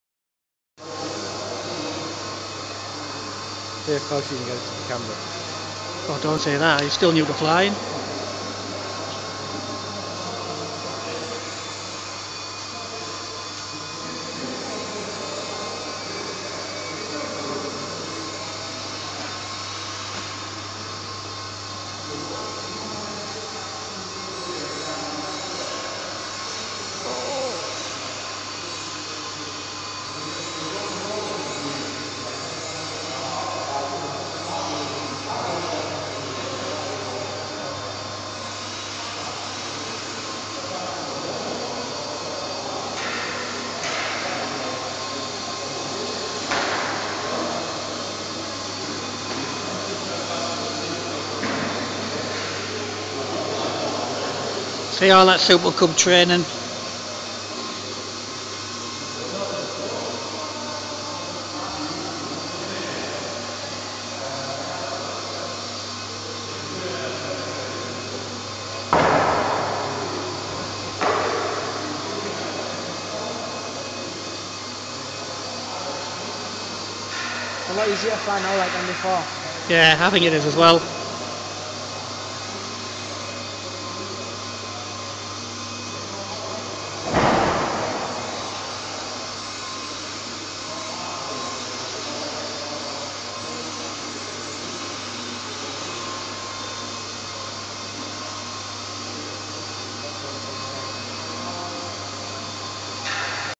Indoor Flying at Nunthorpe School danny flying the pkz vapour.
well Danny has had 4 flights with the supercub and nether flown indoors and has had zero flights with the parkzone vapour.
did very well.